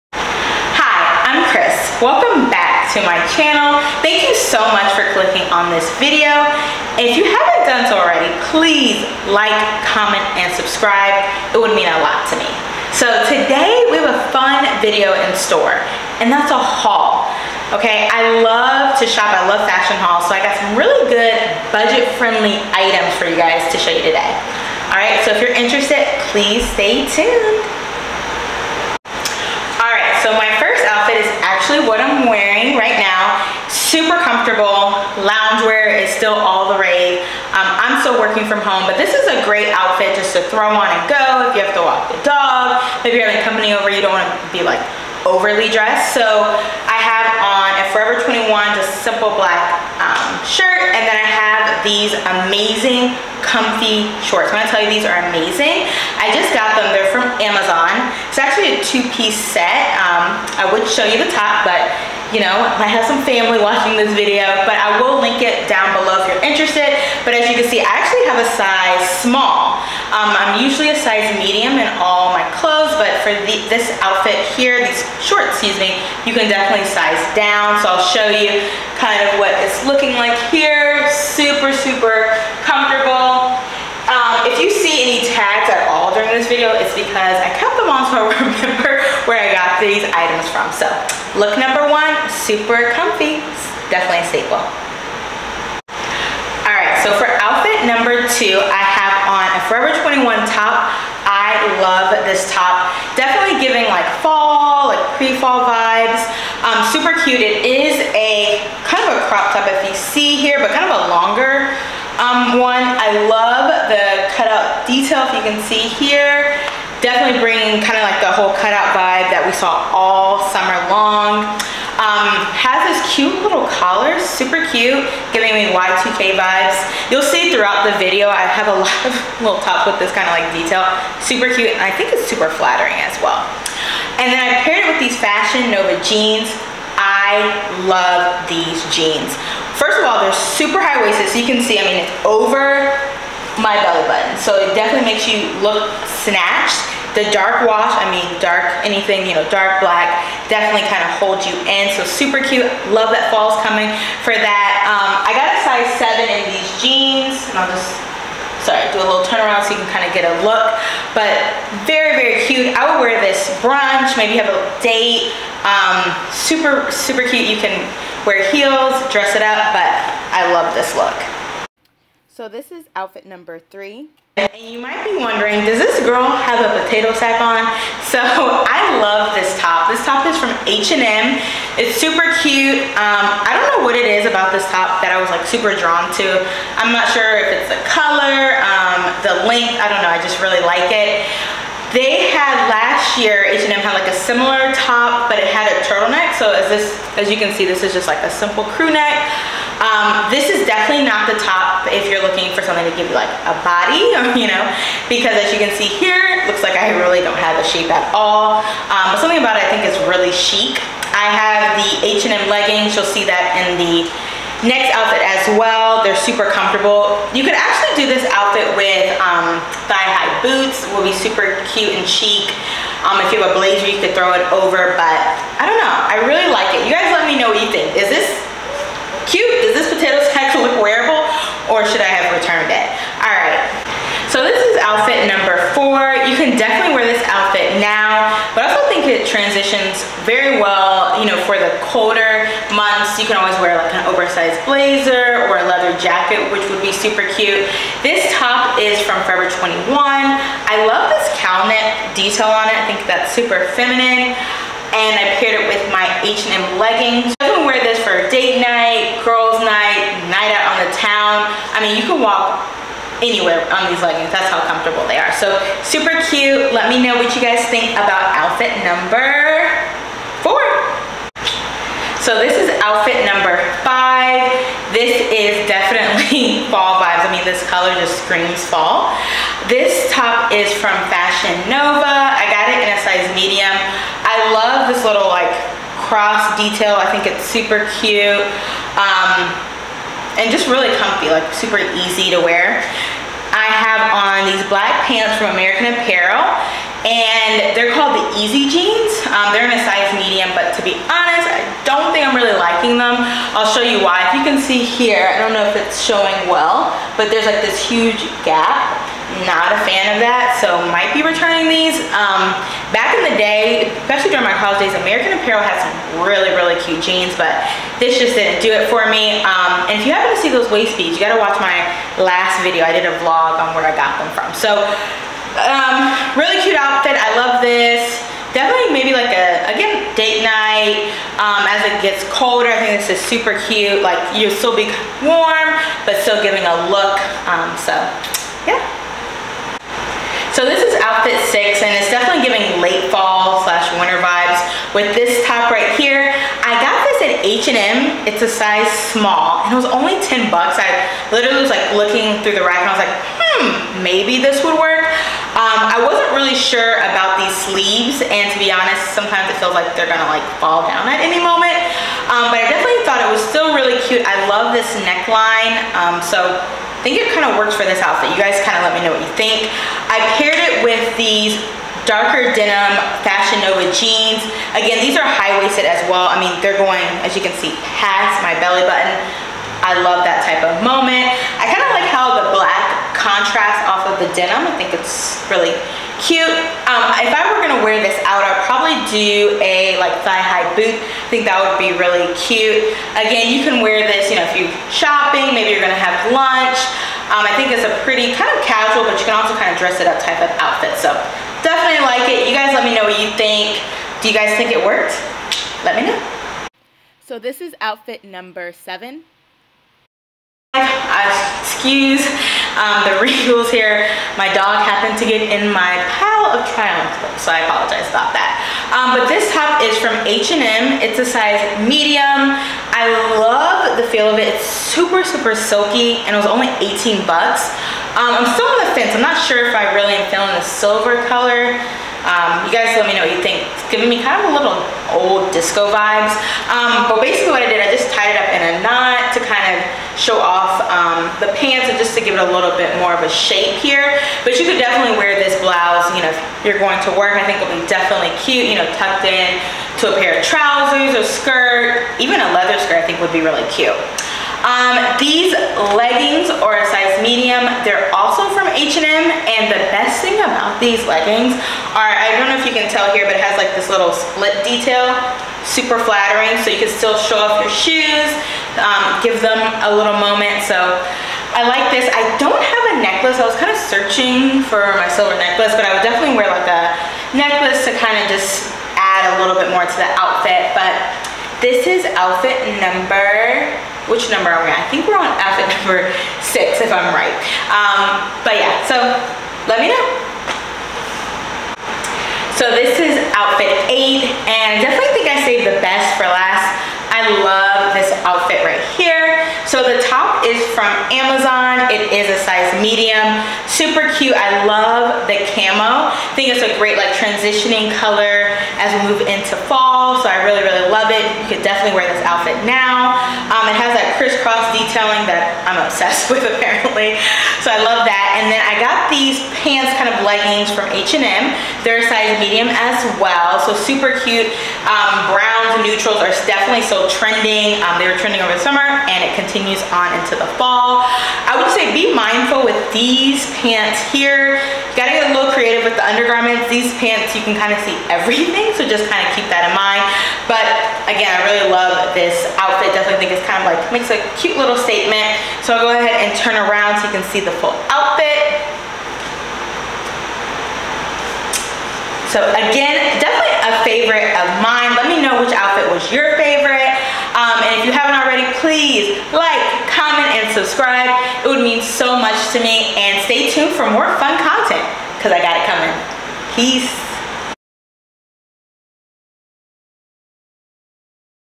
New- in: Try on Haul 2021 - H&M , Fashion Nova and More
New- in: Transition pieces- night- out and casual fits

Outfits

1. Amazon - two piece set